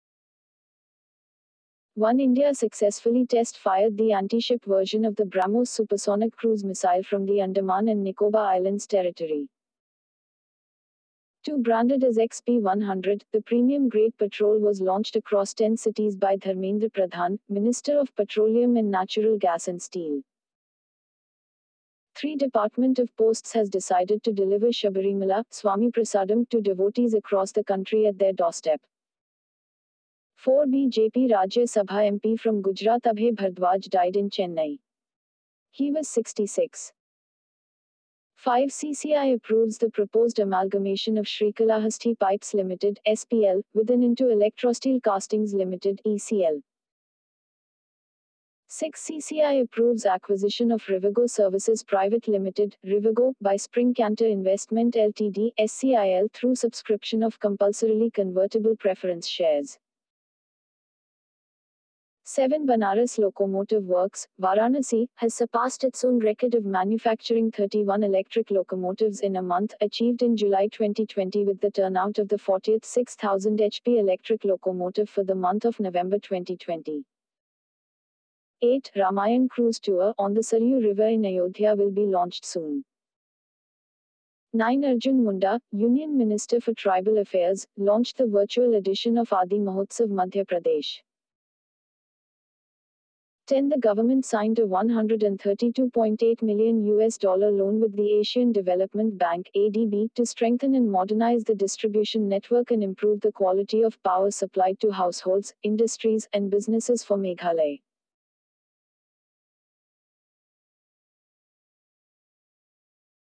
Daily current affairs
1-India successfully test-fired the anti-ship version of the BrahMos supersonic cruise missile from the Andaman and Nicobar Islands territory.

2-Branded as XP100, the premium grade petrol was launched across ten cities by Dharmendra Pradhan, Minister of Petroleum & Natural Gas and Steel.

3-Department of Posts has decided to deliver Sabarimala ‘SwamyPrasadam' to devotees across the country at their doorstep.

4-BJP Rajya Sabha MP from Gujarat Abhay Bharadwaj died in Chennai. He was 66.

5-CCI approves the proposed amalgamation of Srikalahasthi Pipes Limited (SPL) with and into Electrosteel Castings Limited (ECL).

6-CCI approves acquisition of Rivigo Services Private Limited (Rivigo) by Spring Canter Investment Ltd (SCIL) through subscription of compulsorily convertible preference shares.

7-Banaras Locomotive Works, Varanasi, has surpassed it’s own record of manufacturing 31 electric locomotives in a month, achieved in July 2020 with the turnout of the 40th 6,000 HP electric locomotive for the month of November 2020.

8-‘Ramayan Cruise Tour’ on the Saryu river in Ayodhya will be launched soon.

9-Arjun Munda, Union Minister for Tribal Affairs, launched the virtual edition of Aadi Mahotsav- Madhya Pradesh.

10-The government signed a 132.8 million US dollar loan with the Asian Development Bank (ADB) to strengthen and modernize the distribution network and improve the quality of power supplied to households, industries, and businesses for Meghalaya.